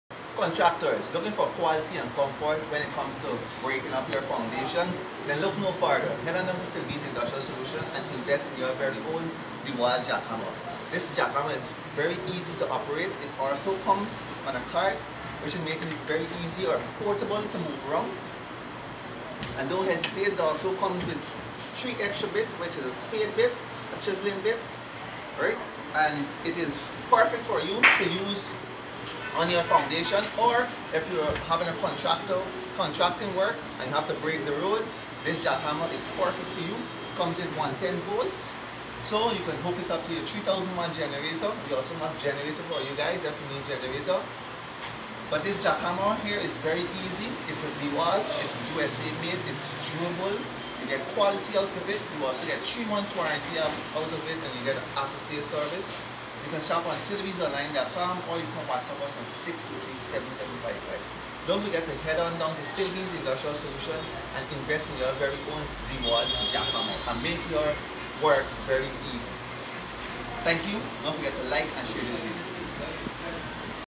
Contractors! Invest Today in a Dewalt Jack Hammer from Silvie's Industrial Solutions
Contractors! Invest today in a Dewalt Jack Hammer from Silvie's Industrial Solutions. Visit us at our 31 High and Hadfield streets location or shop online from our